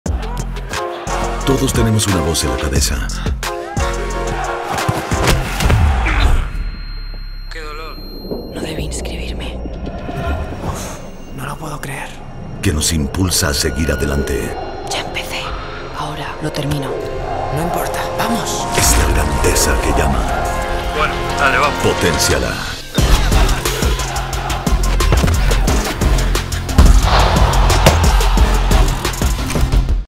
Gatorade - Potencia la voz de tu cabeza
Esa voz en tu cabeza que te impulsa a seguir, es la grandeza, poténciala. Gatorade, hidrata, repone, activa.